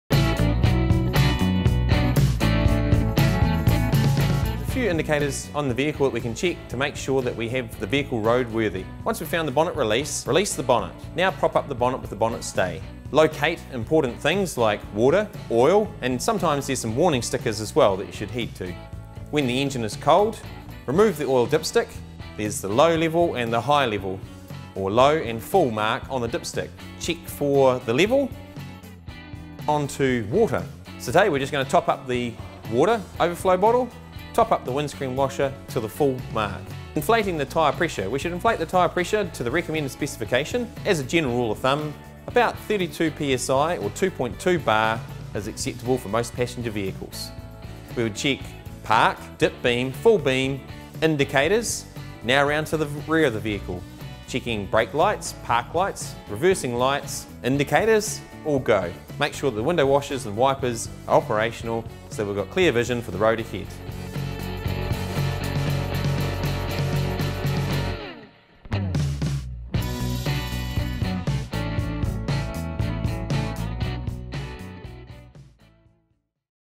How to: do a pre-trip check
Peter Sauer, the head of Automotive Trades at Ara Institute of Canterbury, covers off the basics of a pre-trip check. From making sure your car is road worthy and road legal, through checking your oil and water, down to checking the window wipers are doing the job, watch and learn the basics to check your car is ready for a long distance road-trip.